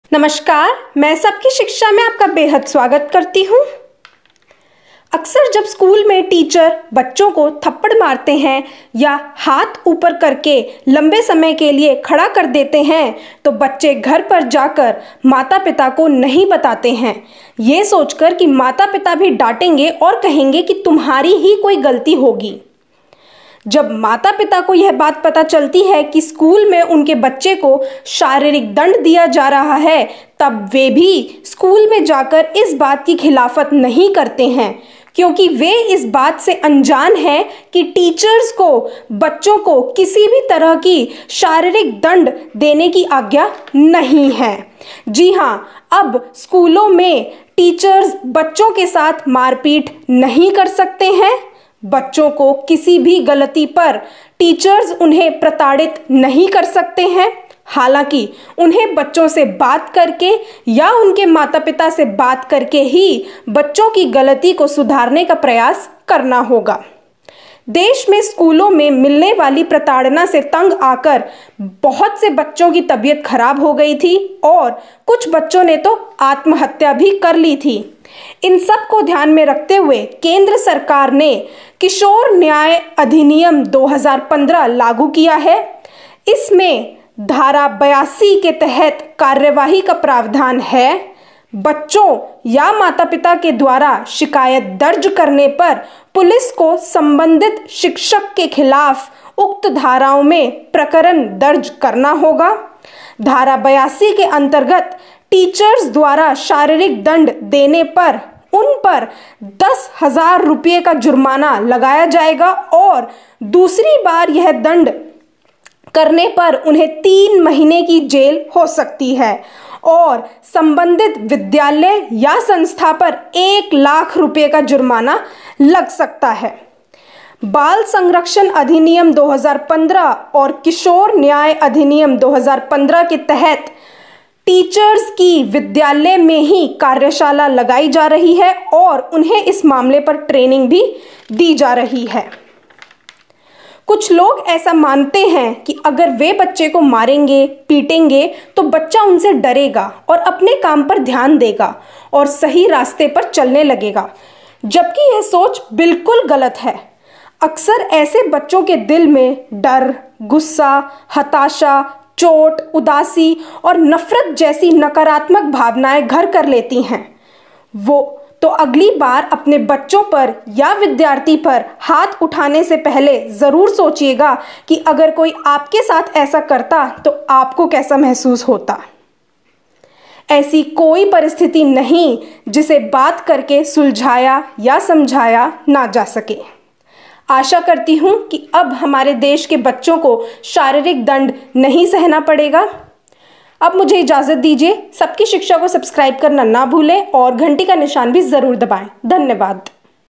अगर टीचर बच्चों को मारे तो क्या करें / बच्चों की स्कूल में होती पिटाई को ना करें नज़रंदाज़
अगर टीचर बच्चों को मारे तो क्या करें

बच्चों की स्कूल में होती पिटाई को ना करें नज़रंदाज़